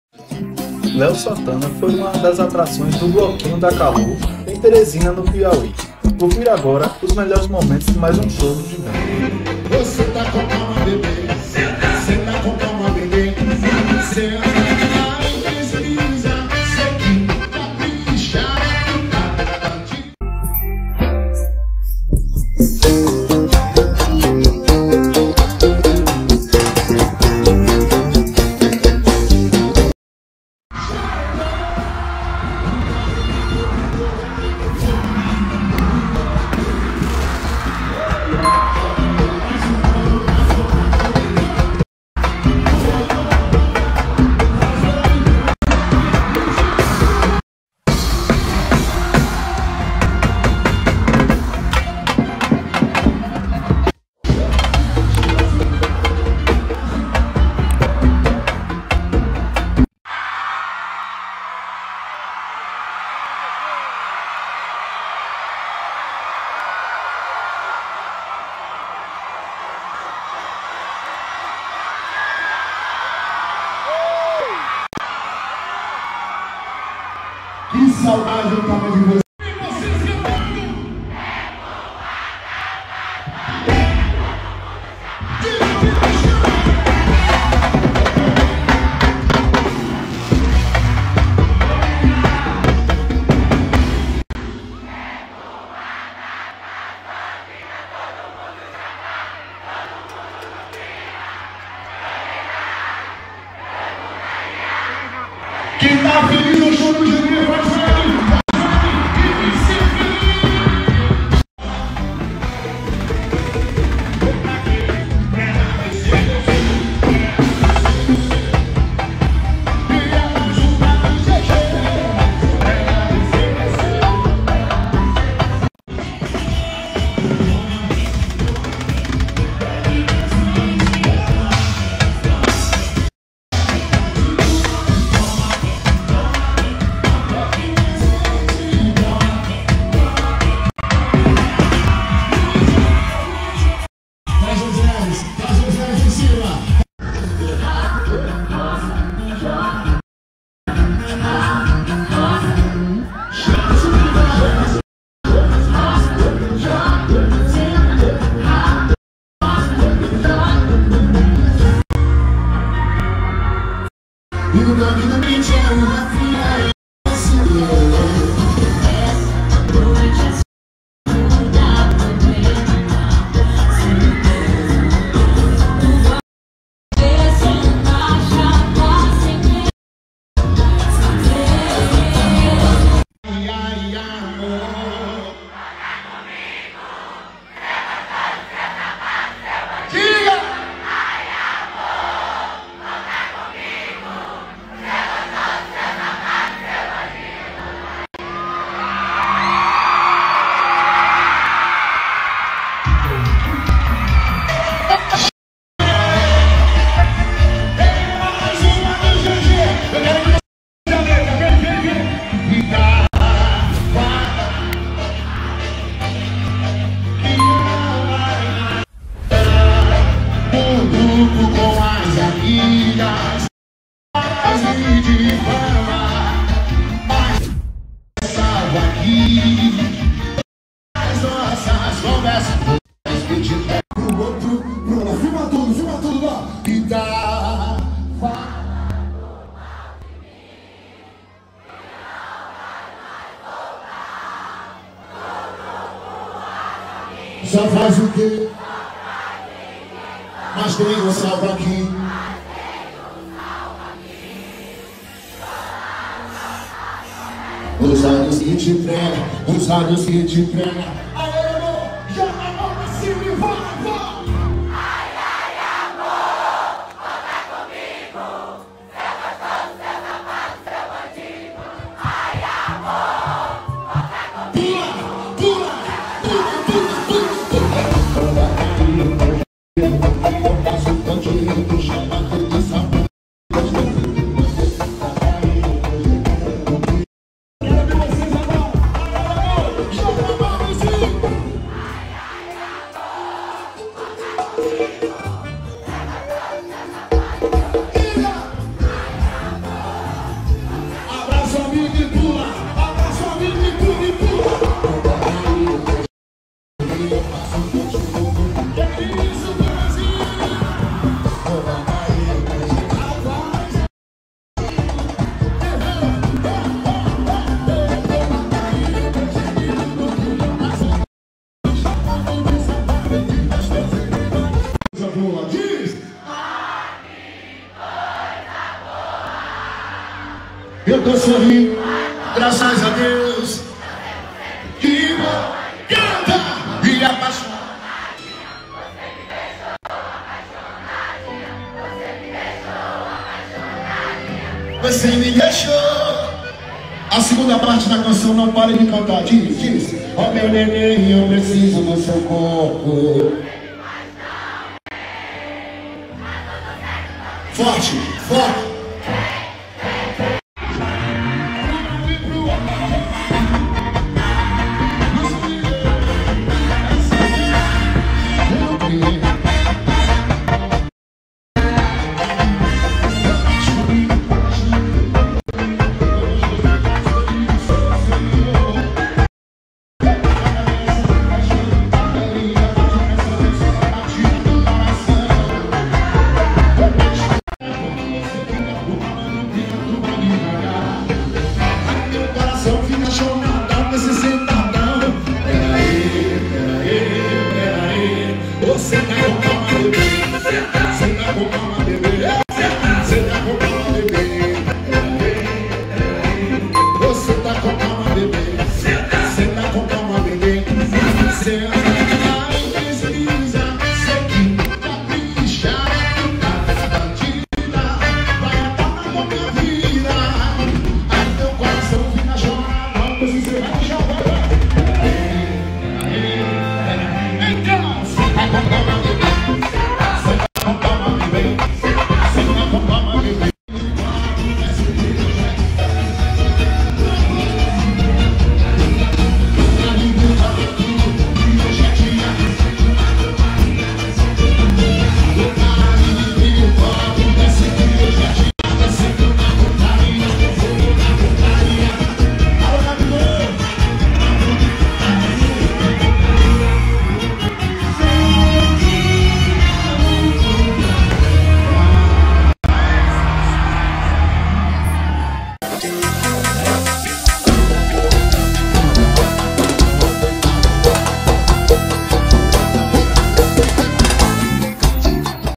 LÉO SANTANA NO BLOQUINHO DA KALOR EM TERESINA/PI - 01/05/2022
O Gigante foi a Teresina/PI e agitou o Bloquinho da Kalor